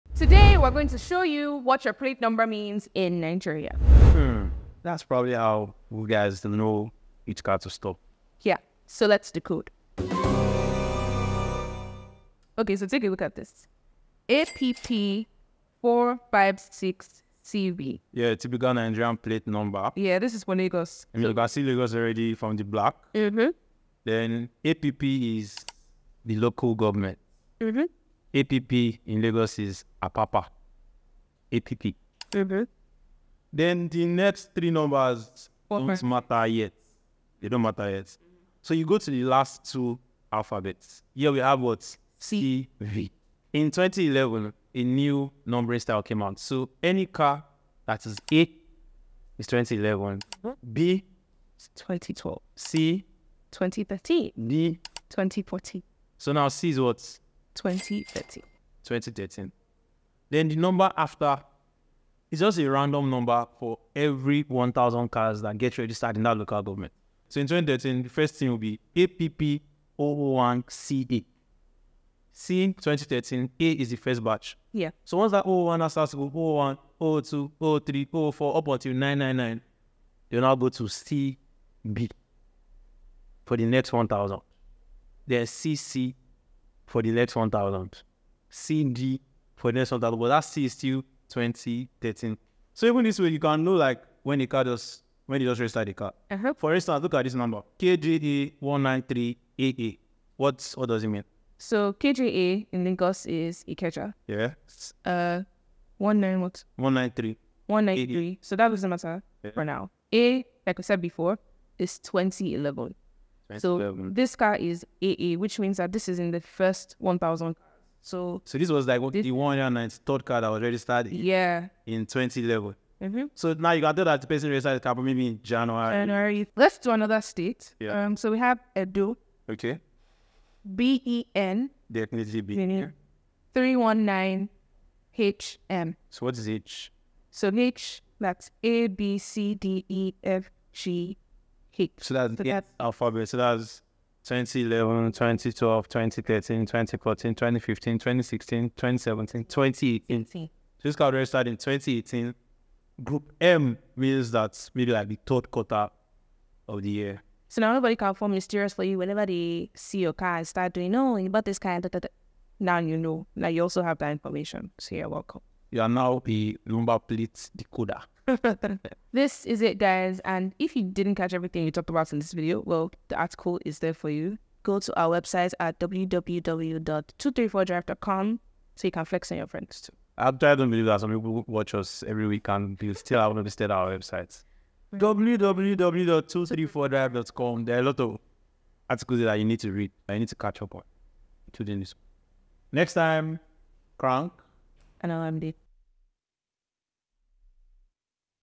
How to DECODE A Nigerian NUMBER PLATE in 10 seconds!
This video shows how to decode the Number plate lettering system in Nigeria although there are a few exceptions for custom plates, this rule generally holds.

Link to our website below ; @